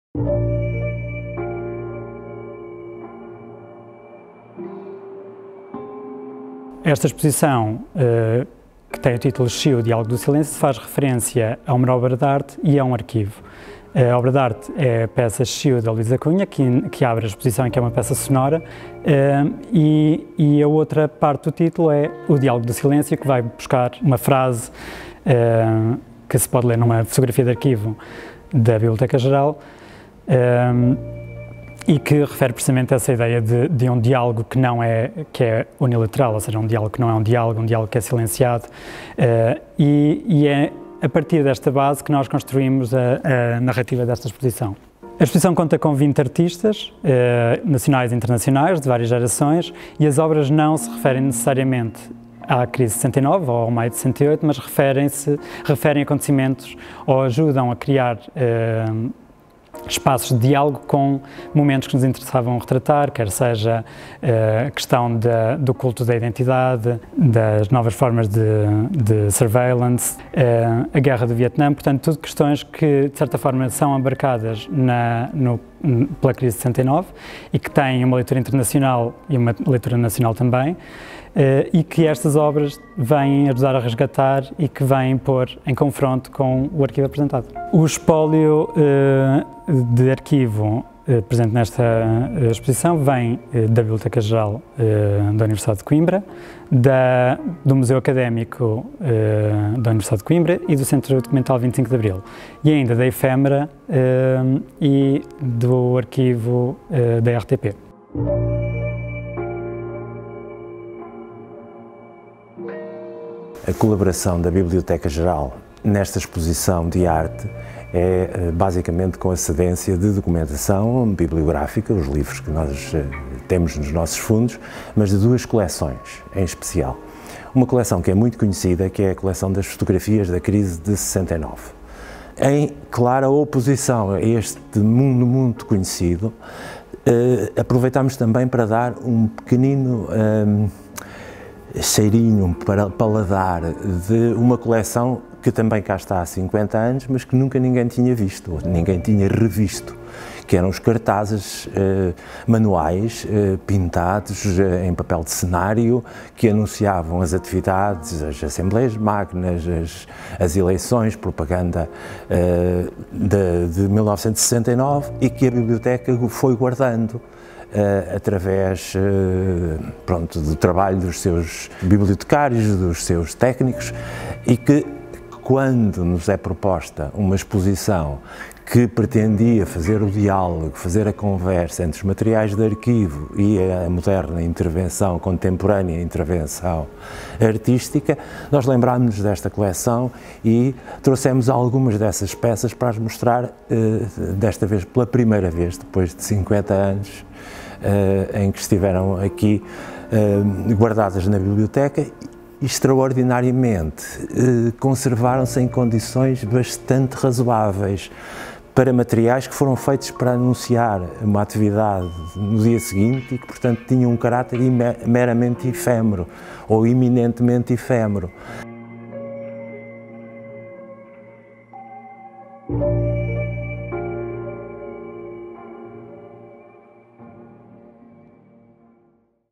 SHIU! O Diálogo do Silêncio
A Sala da São Pedro da Biblioteca Geral da UC acolhe, até dia 29 de dezembro, a exposição SHIU! O Diálogo do Silêncio, no âmbito da programação do Anozero ’19 – Bienal de Arte Contemporânea de Coimbra.
Partindo dos acontecimentos da crise académica de Coimbra de 1969, esta exposição procura revisitar os momentos de tensão e manifestação dos estudantes contra o regime do Estado Novo, ao mesmo tempo que explora questões da atualidade.
Destacando o papel que o material de arquivo pode assumir no contexto de uma exposição de arte contemporânea, procura estabelecer-se afinidades entre obras de arte e a documentação apresentada, permitindo um desdobramento de temas históricos, sociais e políticos que marcaram os últimos cinquenta anos.